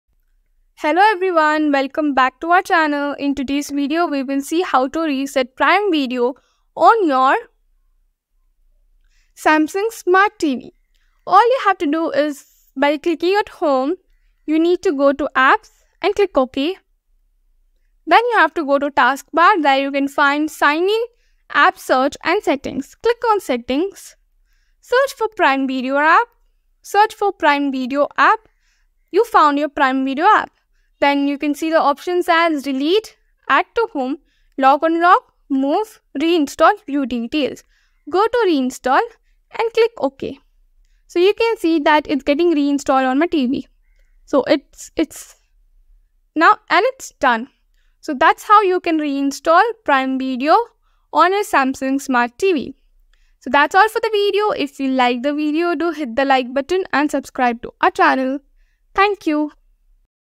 How To Reset Prime Video App On Samsung Smart TV
In this informative video, we will guide you through the process of resetting the Prime Video app on your Samsung Smart TV. Whether you are experiencing technical issues or simply wish to refresh the app, our step-by-step instructions will ensure a smooth reset.

0:00 Introduction
0:16 Step1 :- Go To Apps
0:26 Step2 :- Search Prime Video App
0:43 Step3 :- Click Reinstall

Join us as we delve into the world of Samsung TV, showcasing its impressive features and functionalities.